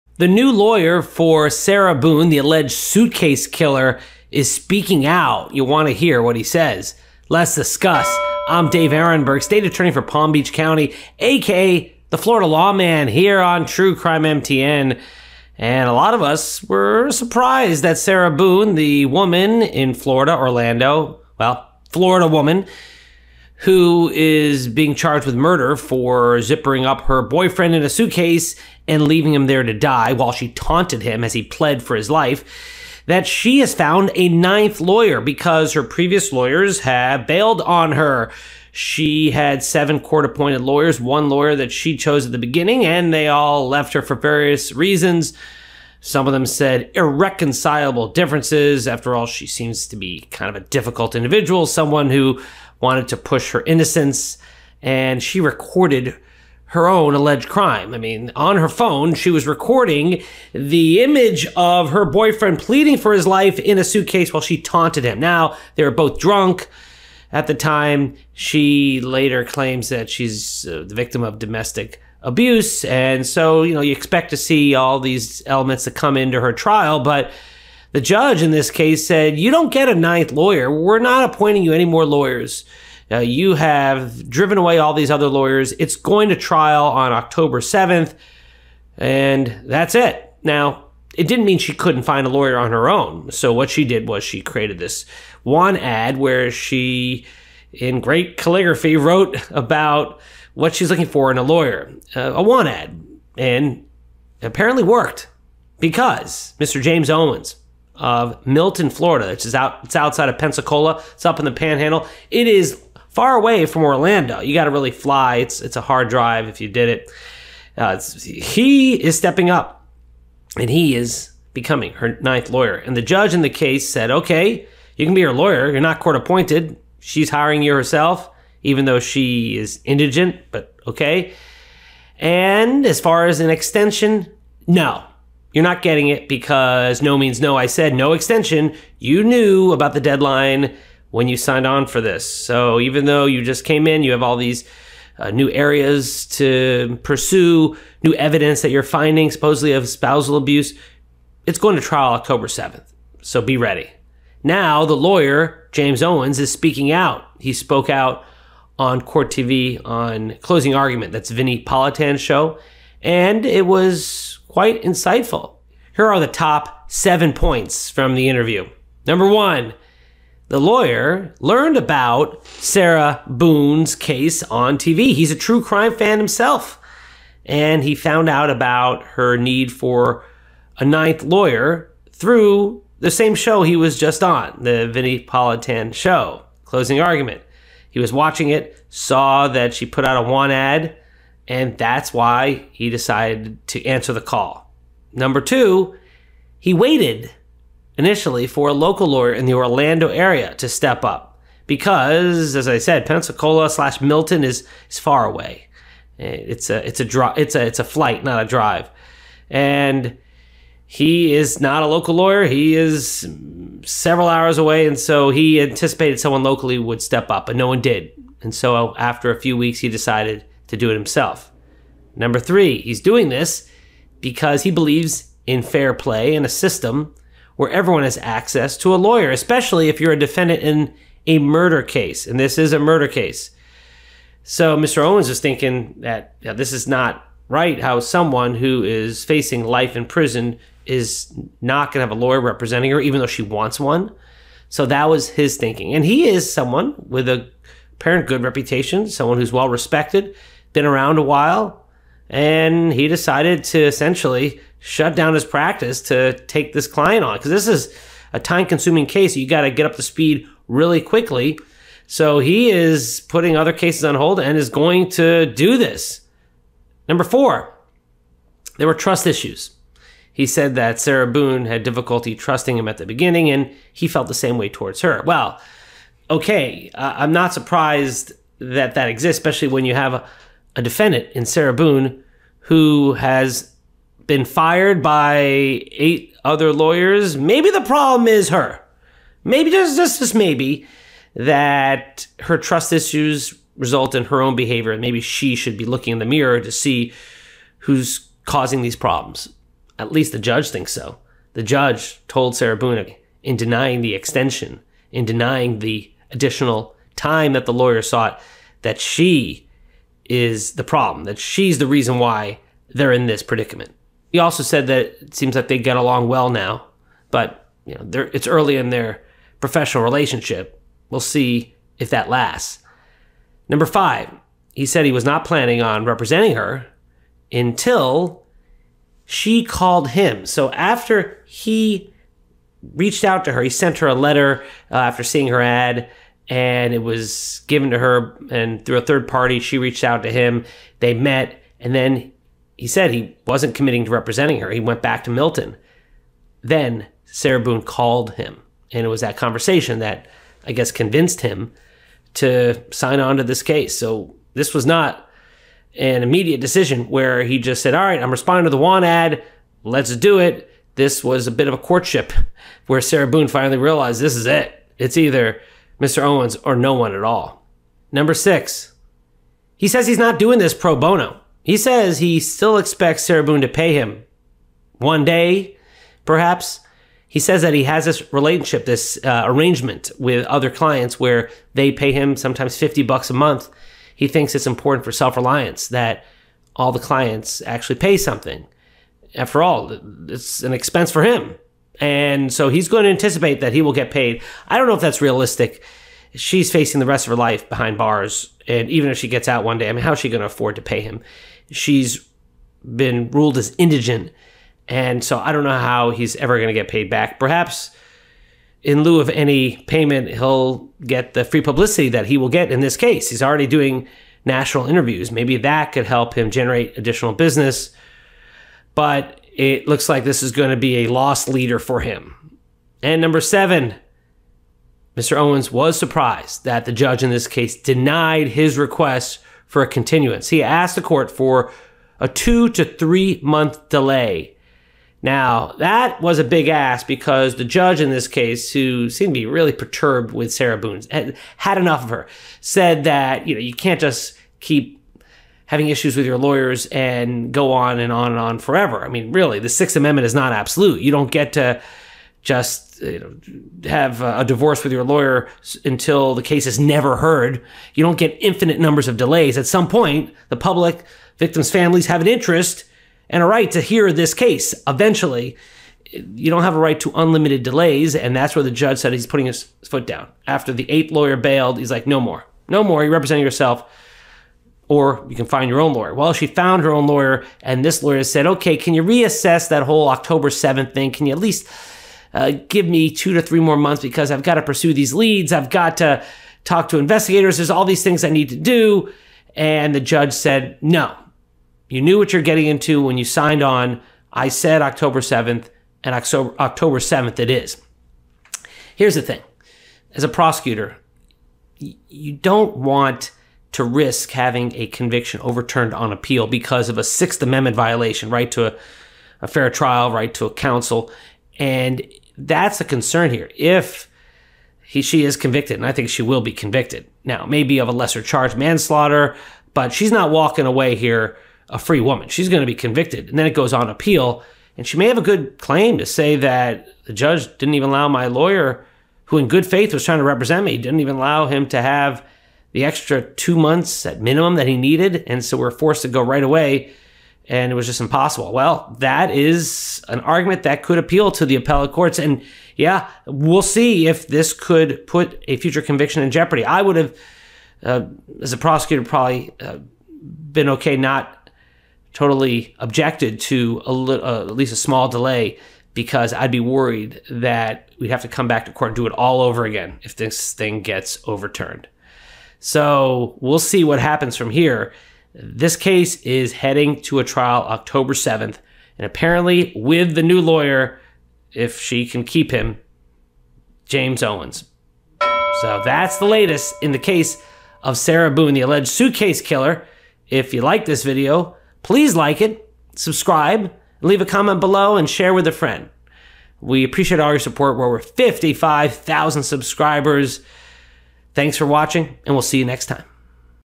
Boone’s NEW Lawyer SPEAKS OUT on His Own Client
Sarah Boone's new lawyer, James Owens, speaks out! The 9th and latest defense lawyer for the accused "suitcase" murderer, Sarah Boone, gave an insightful interview about why he decided to represent the notorious defendant, whether he is getting paid, and his reaction to the judge's decision to deny his request to delay the trial. Prosecutor Dave Aronberg, AKA the Florida Law Man, reviews the 7 biggest takeaways from the interview, and what it all means for Boone's chances in this nationally watched case that it heading to trial on October 7th.